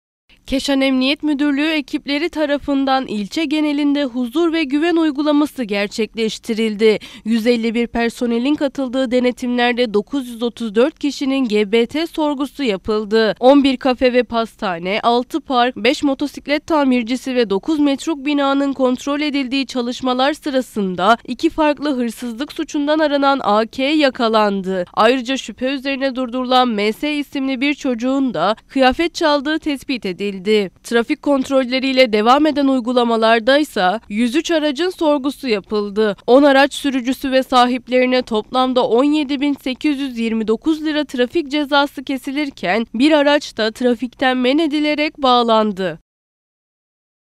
KEŞAN'DA HUZUR VE GÜVEN UYGULAMASI
Edirne’nin Keşan ilçesinde polis ekipleri tarafından gerçekleştirilen huzur ve güven uygulamasında; aranan bir kişi yakalanırken, 17 bin 829 lira trafik cezası kesildi.